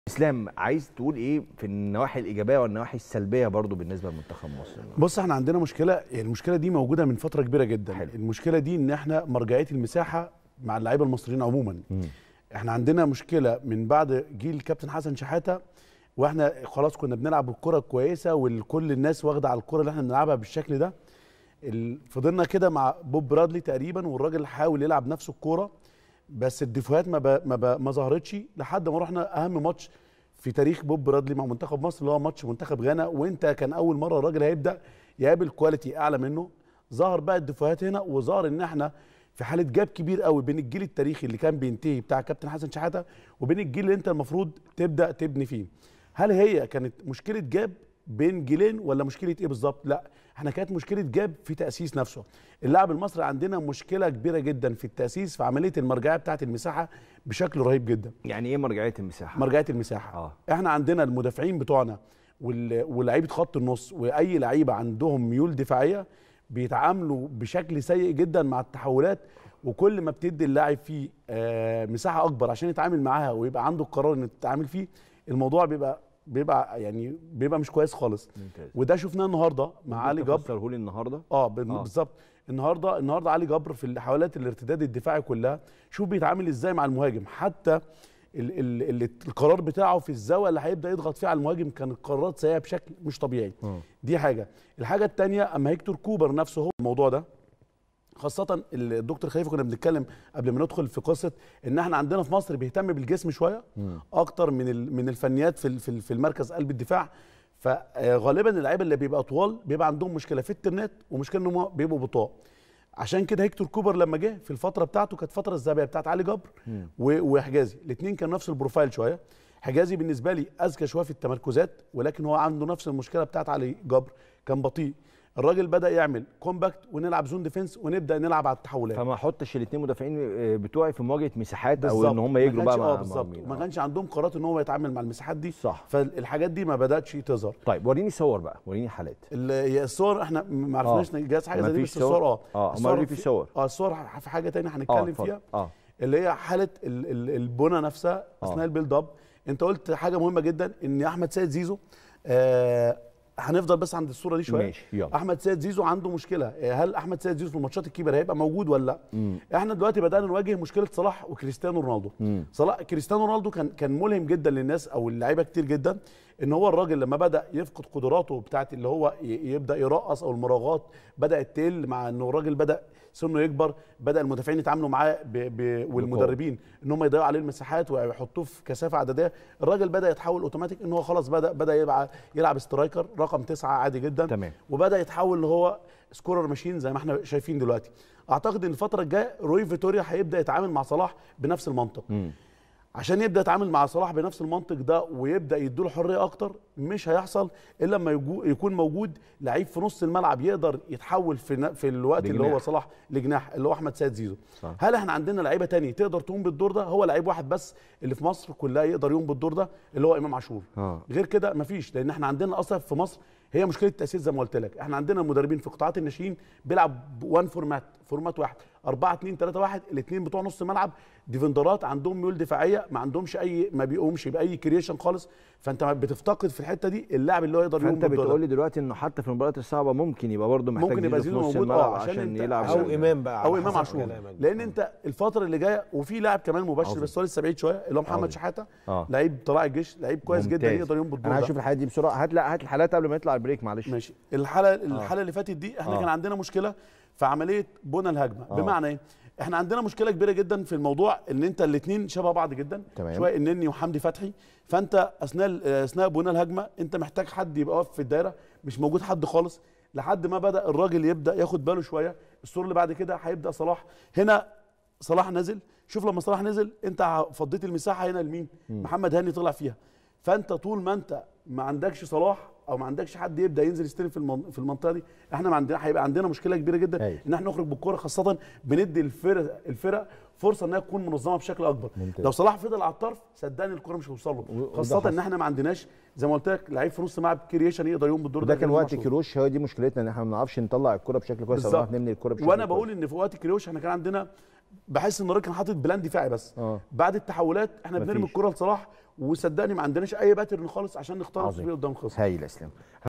تعرف على عيوب اللاعب في بناء الهجمة مع منتخب مصر 🔥🔥تعليق اسلام علي على تحليل اداء منتخب مصر التكت
"تعرف على عيوب اللاعب في بناء الهجمة مع منتخب مصر"🔥🔥تعليق اسلام علي على تحليل اداء منتخب مصر التكتيكي امام سيراليون  💣💣